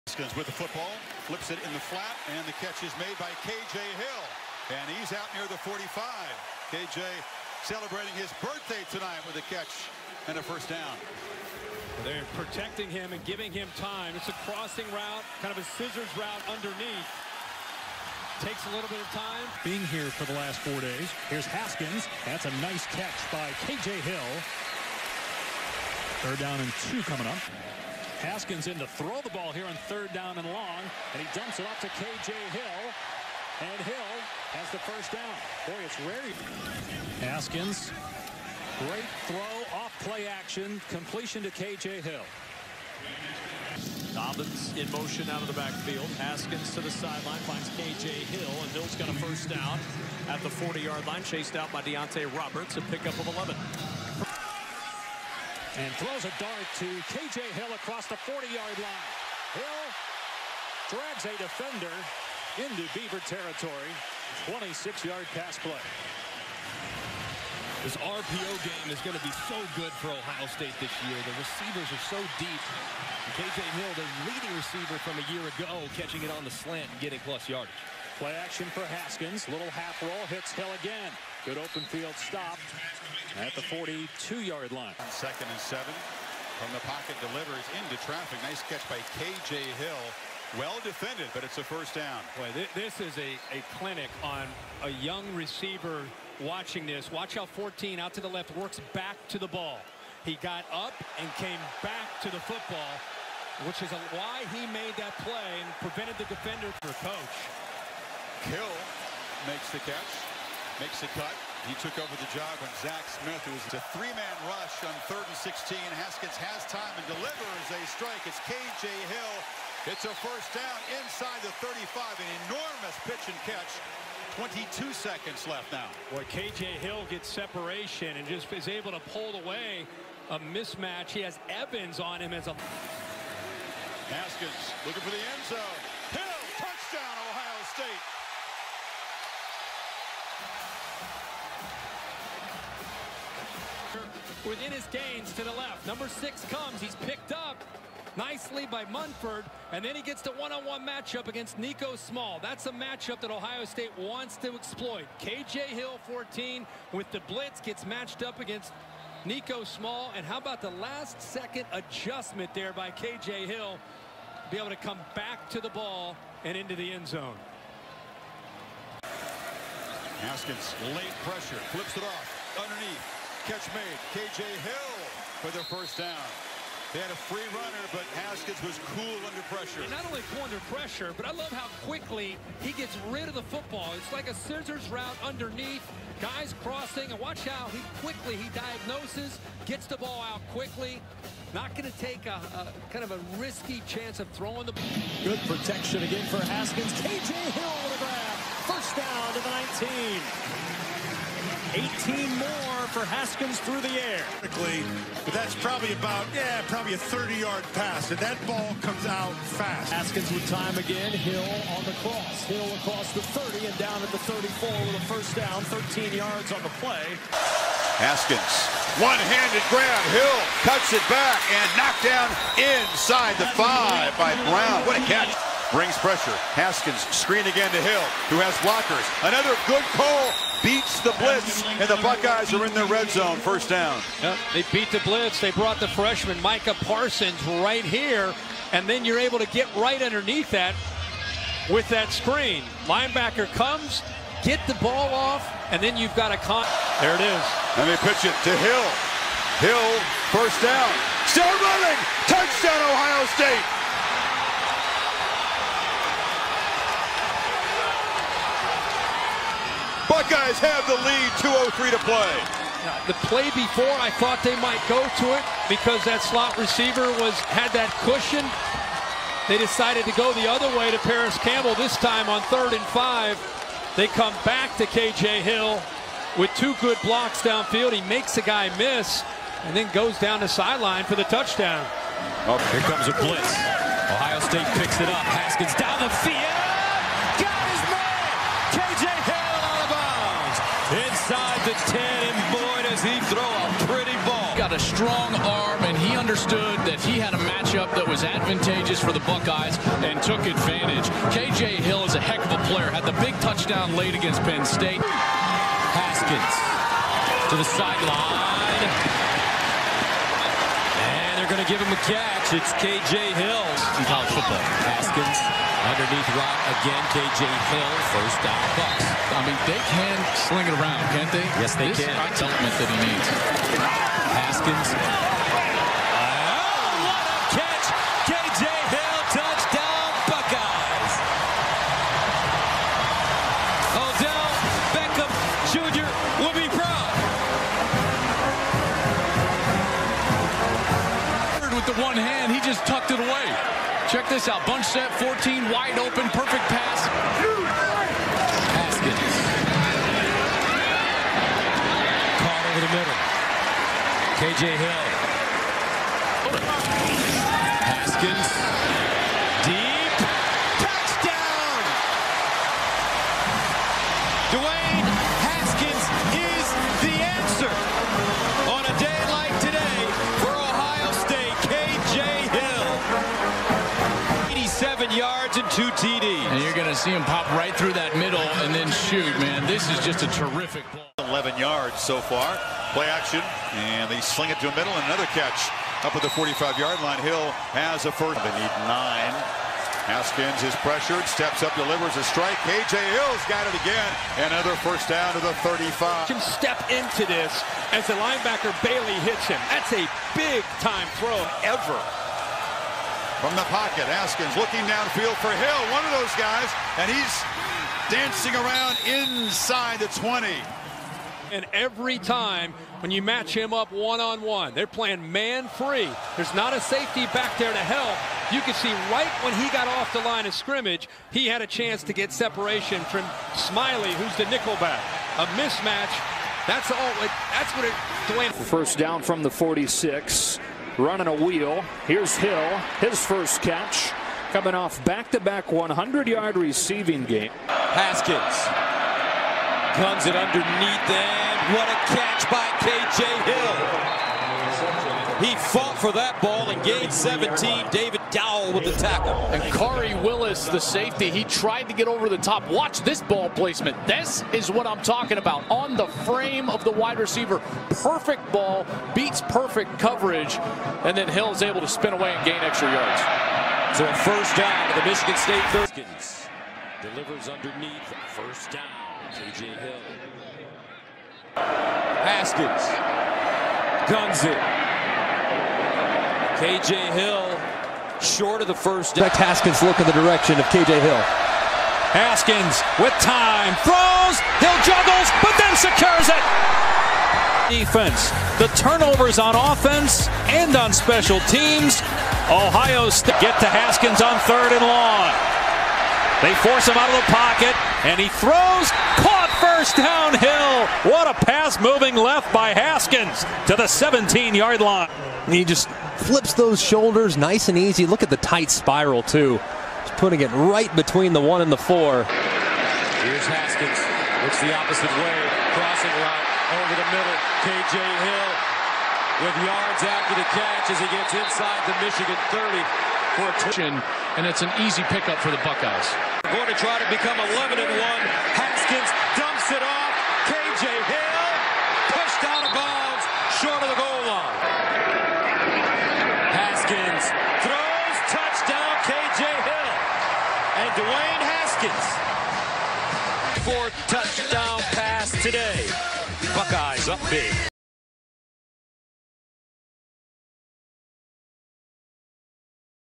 K.J. Hill COMPLETE 2018 Highlights
Highlights of Ohio State receiver K.J. Hill during the 2018 college football season